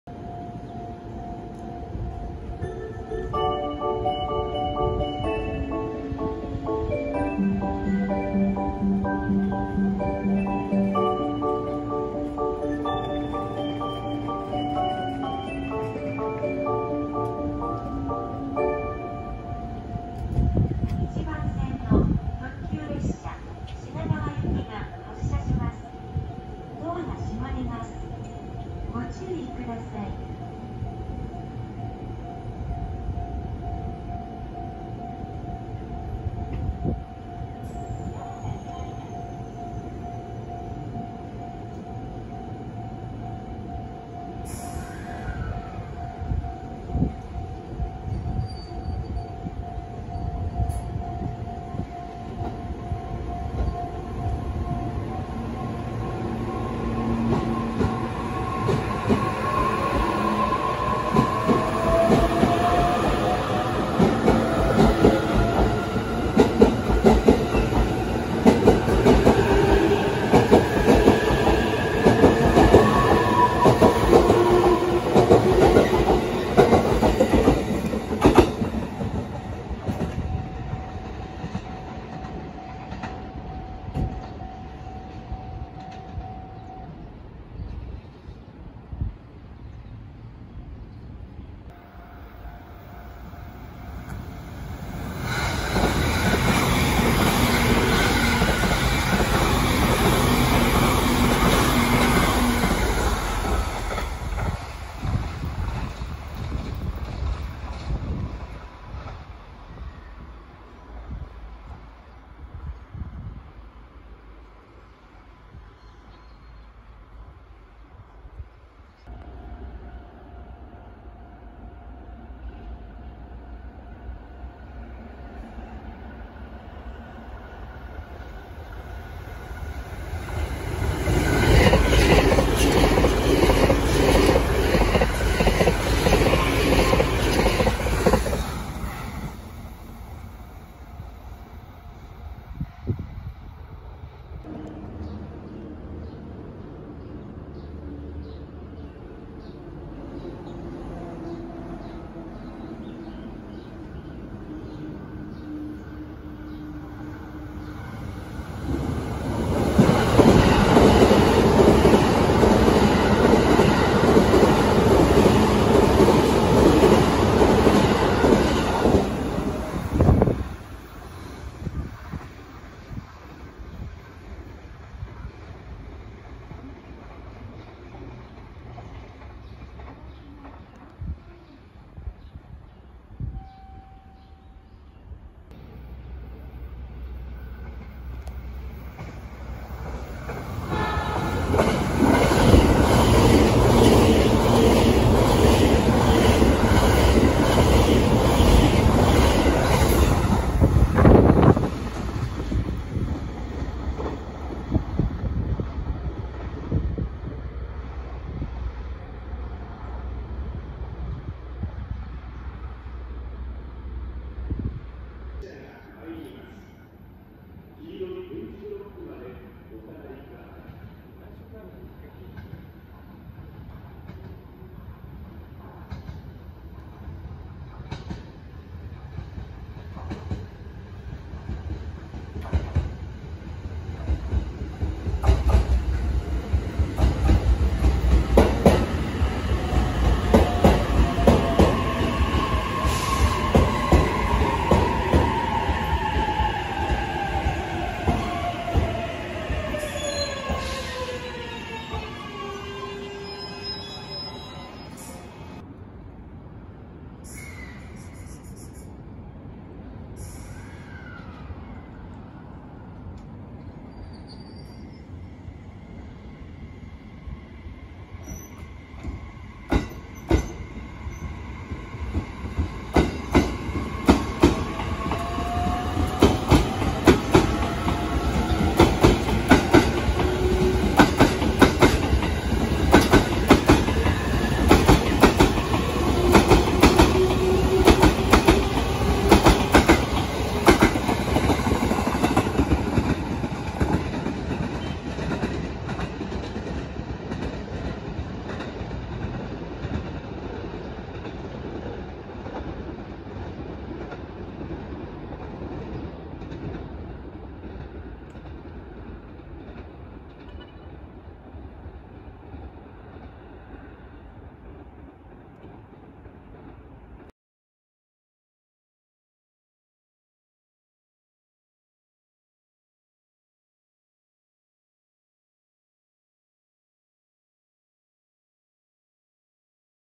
常磐線 福島県を走る E657系ひたち号特集！
福島県浜通りを走る特急ひたち号の発着と通過動画をお届けします。